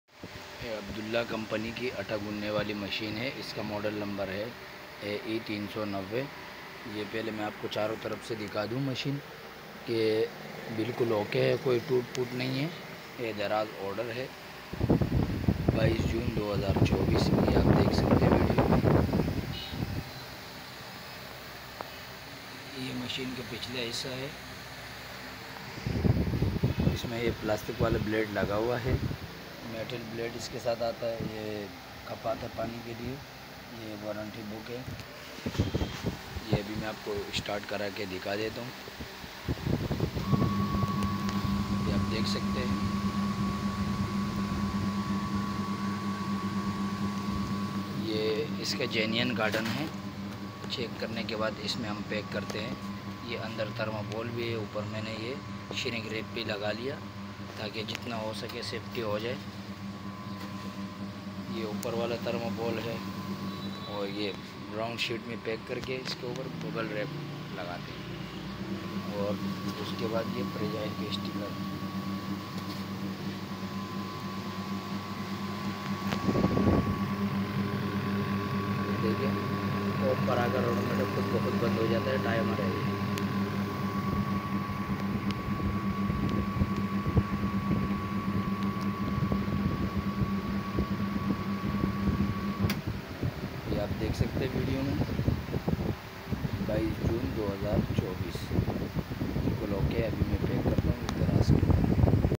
Abdullah Aata Machine Latest Hightop model AE-390 of 3.5kg - Orion Engg. Daraz Order June 22, 2024
Latest model of Abdullah Dough Maker Kneader 3.5 kg AE-390 | Checking Video of 2024 for Daraz Order

ABDULLAH aatay wali machine Latest model AE390 - Dough Kneader - Aata Maker 3.5 kg - Orion Engg. Co.

Pakistan's Best Dough Kneader Machine
ABDULLAH Atta Machine of 3.5kg LATEST model with Low Sound Level
High Top model AE-390
Complete with Stainless Steel Bowl, Metal Blade, Plastic Blade & Measuring Flask for mixing Water
Checking and Packing Video of 22Jun24 for Daraz Order#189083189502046

Easily Kneads dough within 3 to 5 minutes

Machine is 100% OK, no damages and is working 100% fine. Checked properly and then packed safely and handed over to Daraz Hub Karachi by us.

Orion Engineering Company
Facebook.com/OrionEngineeringCompany